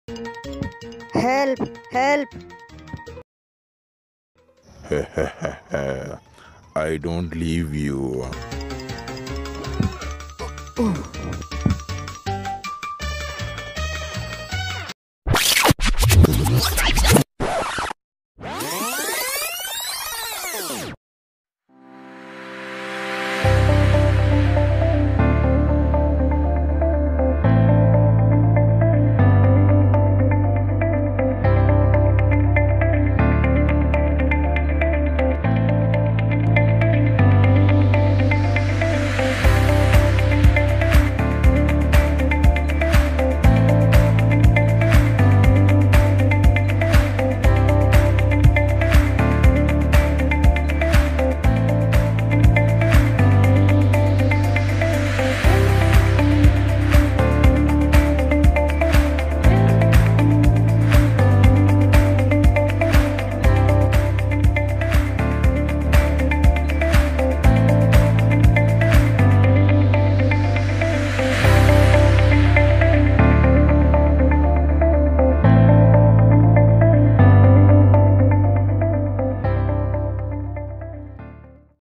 The funny 'onion' .
The present situation in India .Onions are too costly to buy for the people